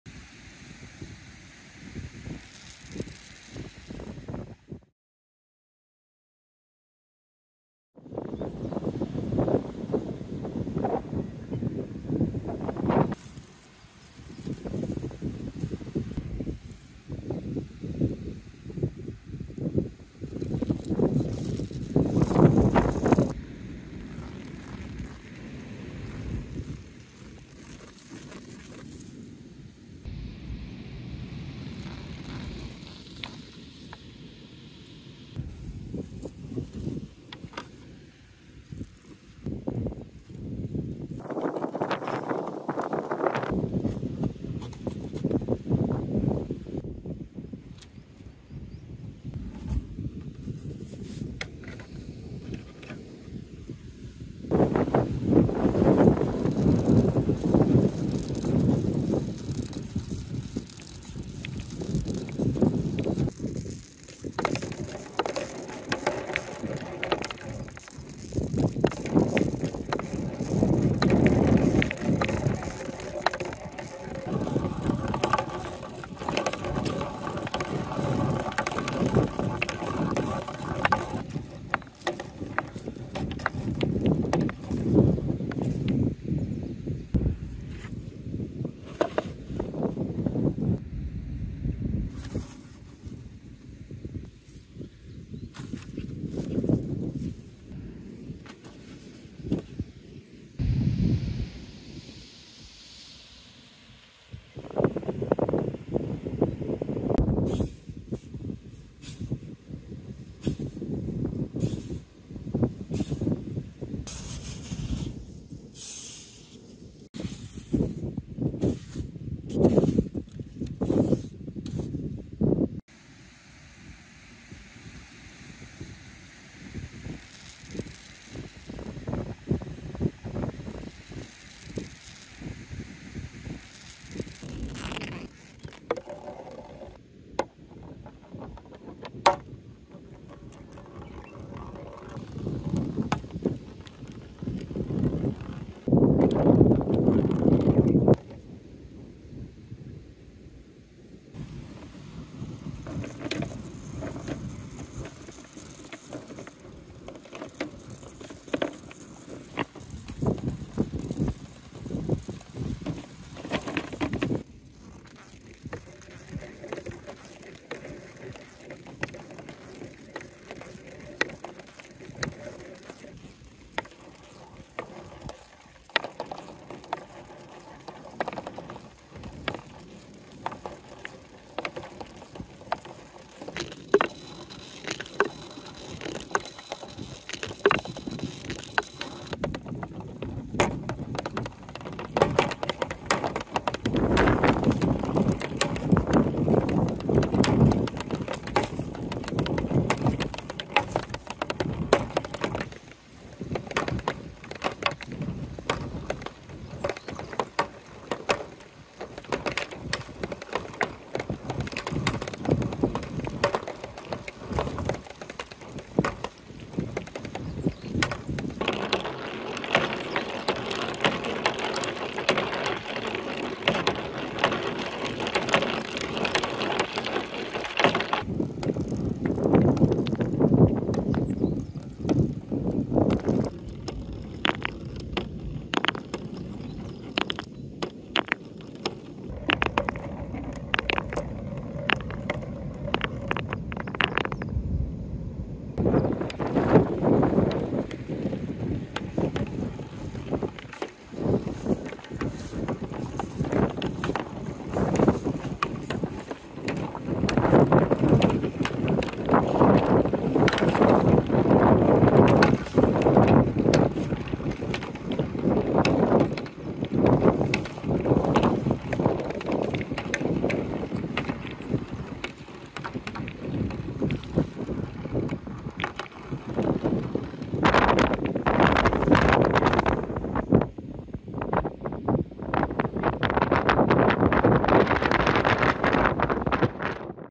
Marble Run Sandy Beach ☆ Bellows Slide + Gutter Sand Hill Course
Marble run on the beach. A bellows slope is set up on a sand hill to create a long course.
It's an impressive course where the balls roll with force from a high place.